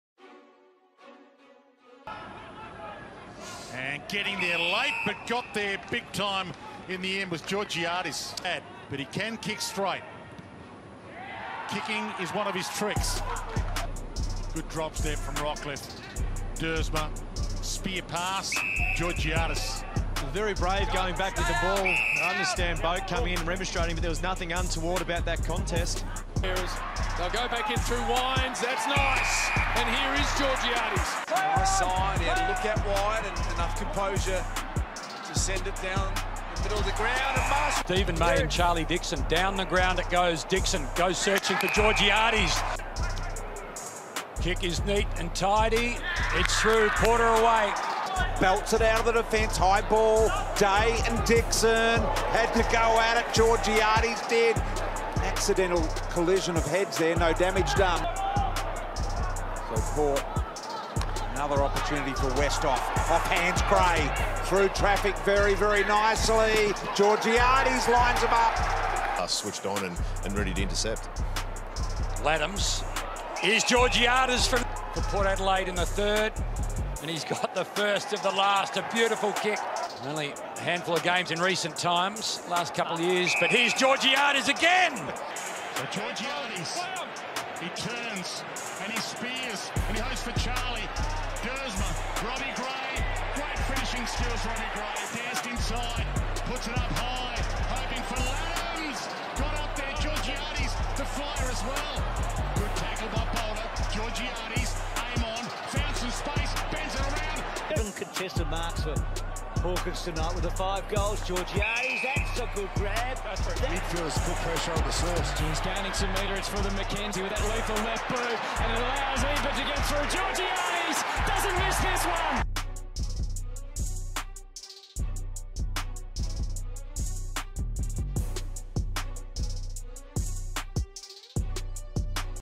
Mitch Georgiades 2020 Highlight Reel
Games: 7
Averages: 8.3 disposals, 2.9 marks, 2 tackles, 1.9 inside 50s, 1.1 goals, 0.4 goal assists per game.

Beat by Unknown Instrumentalz (youtube)